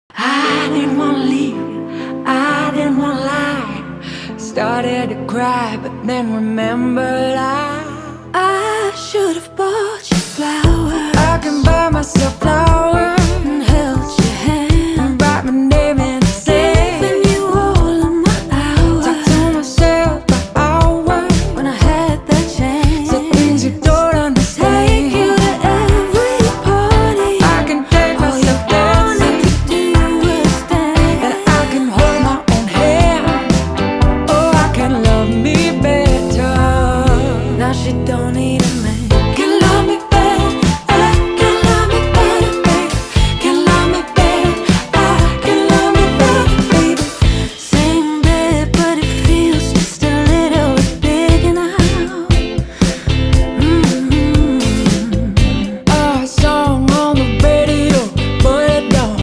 Painting Flowers with Saucepan ⚘️
In this short video, I will show you how to paint flowers with Saucepan.  ⚘️